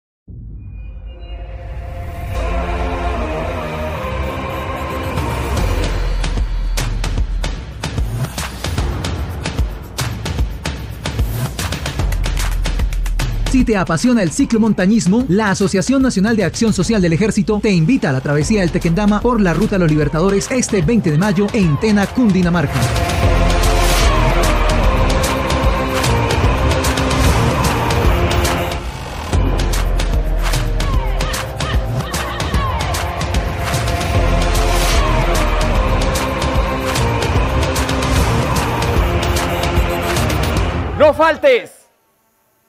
Travesía del Tequendama por la ruta de los Libertadores
Todo listo para la gran travesía del Tequendama por la ruta de los Libertadores en Tena Cundinamarca este 20 de mayo. ¡No faltes!